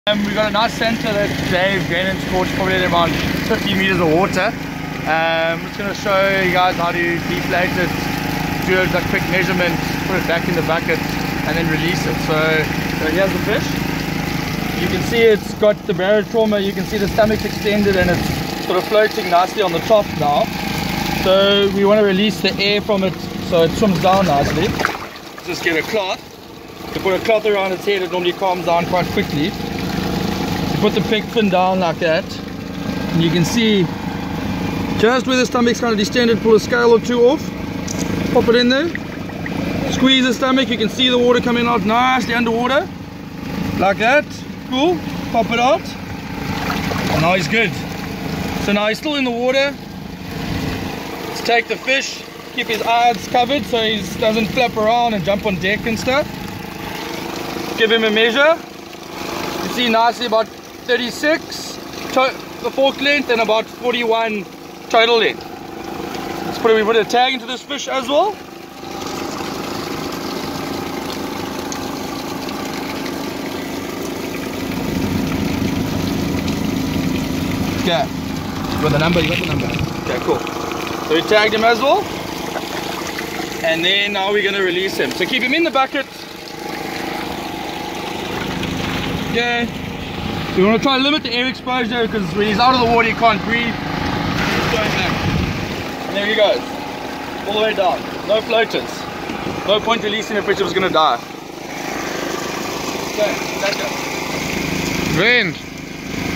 Degassing a basterman using a hypodermic needle (correct needle placement)
An educational video on how to correctly release excess air using a hypodermic needle from a fish brought up from depth and showing signs of barotrauma. Please always use a bucket as it allows the fish to breathe while you are working on it.